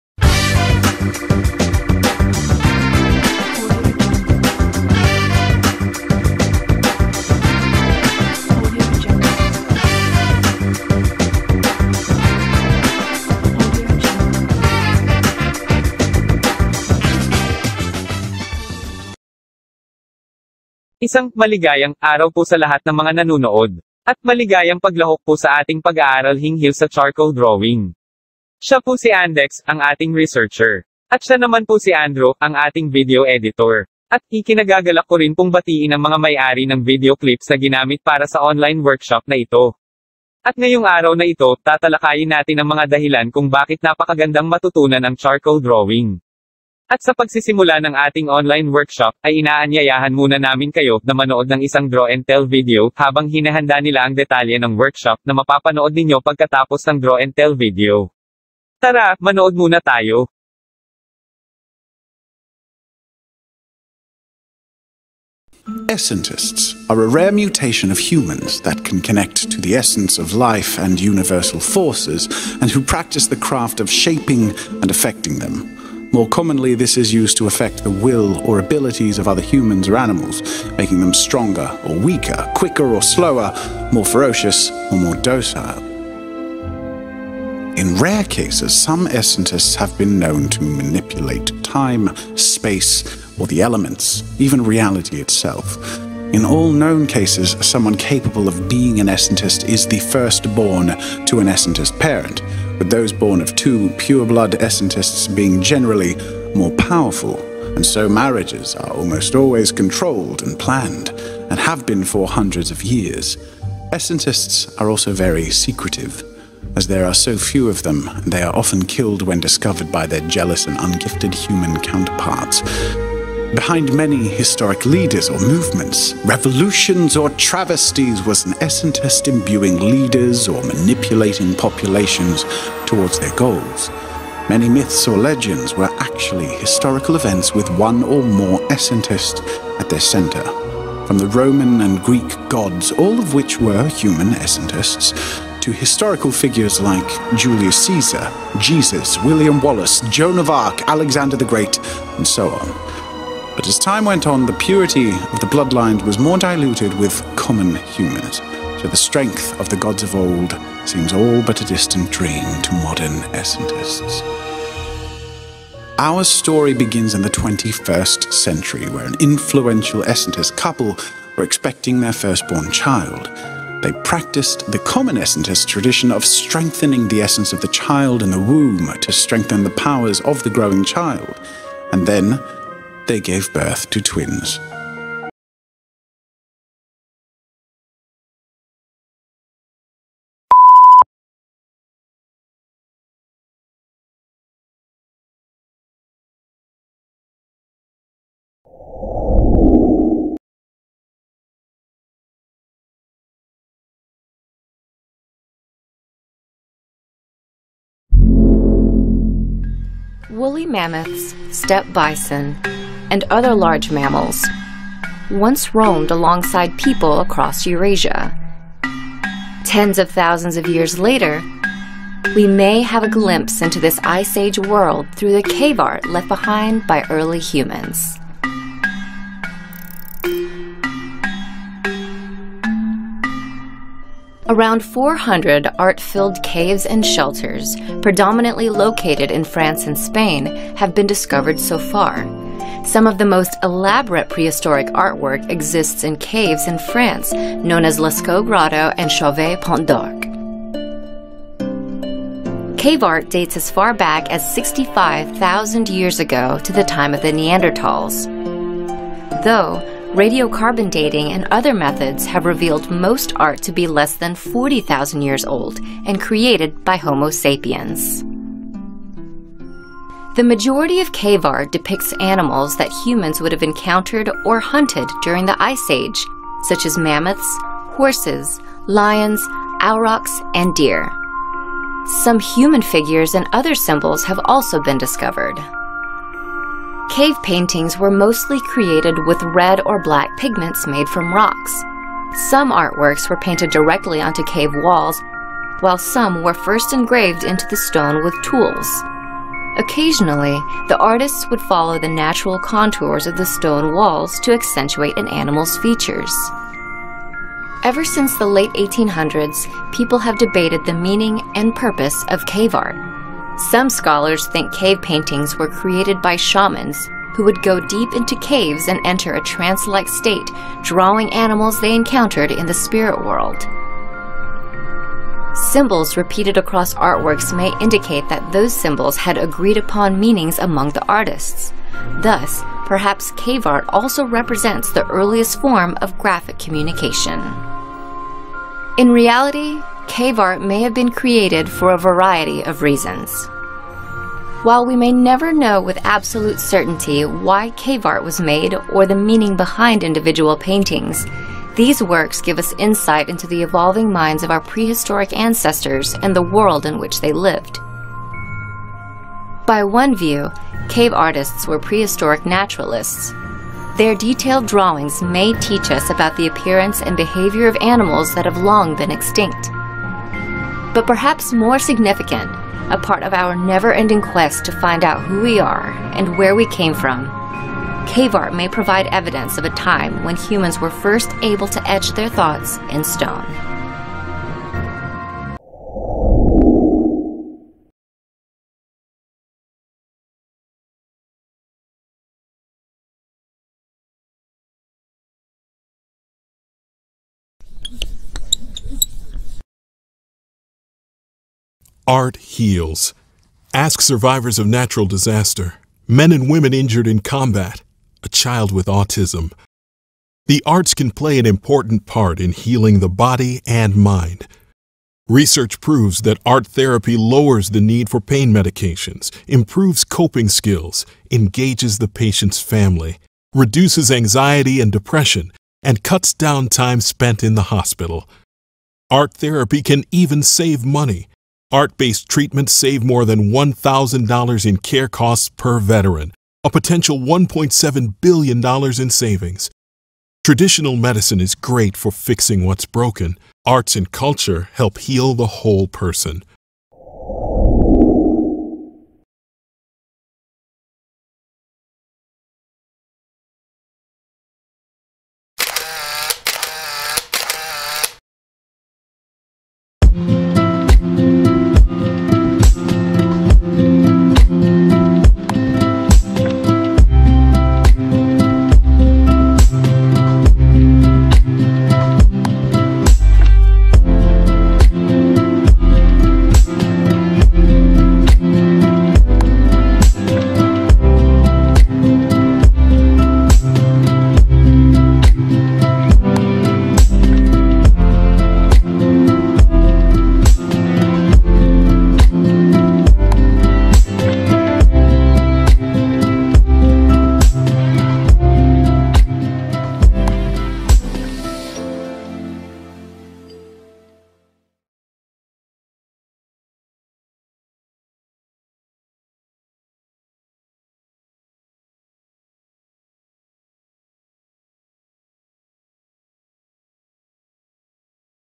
Charcoal Drawing Workshop Part-1 (Philippines)
This Charcoal Drawing Workshop is an annual program of Museo Bulawan held every summer season.